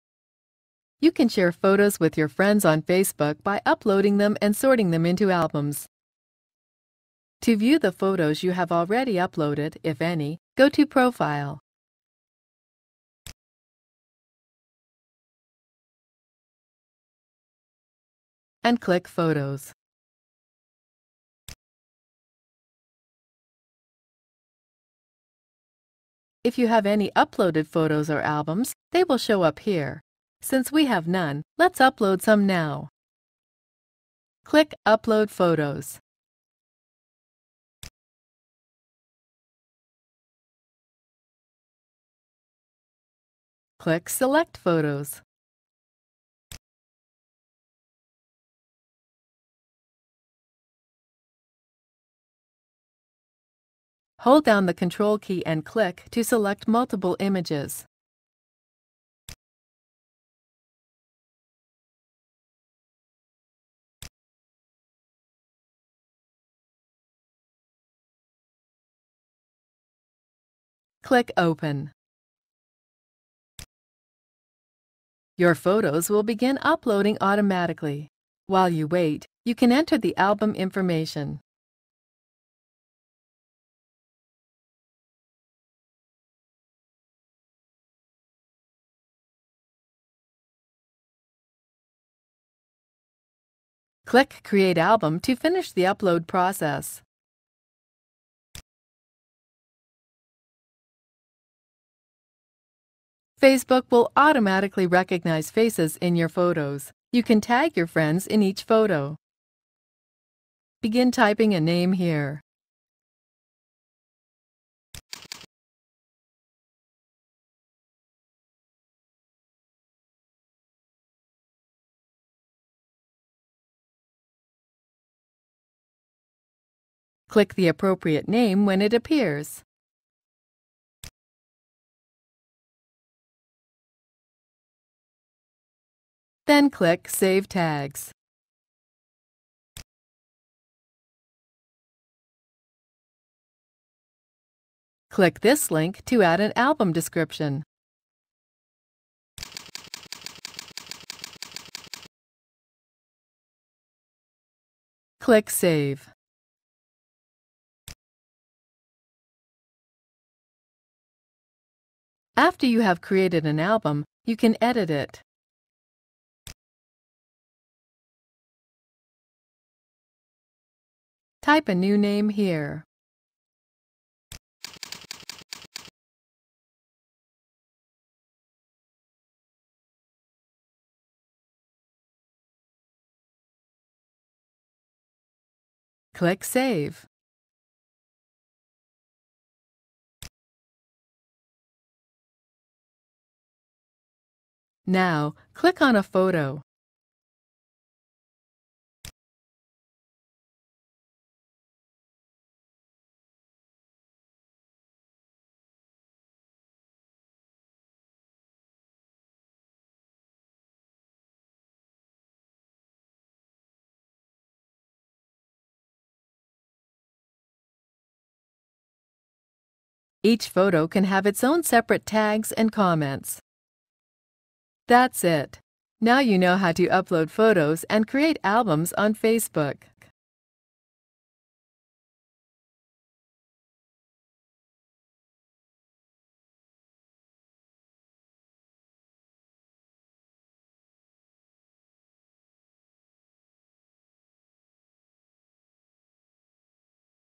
How to manage your photos on Facebook
• Log in your Facebook account.
• You can share photos with your friends on Facebook by uploading them and sorting them into albums.
• Go to Profile. Click on Photos. If you have any uploaded photos or albums, they will be shown.
• Click on Upload photos. Browse them and click on Open button. Your photos will be automatically uploaded. It can be done for a new album or in existing group of photos.
• Facebook recognizes faces in photos. You can tag faces in your photos. You can tag your friends in each photo. Click on Save tags.
• Click on Edit Album tag to edit album specifications. Click on Save button.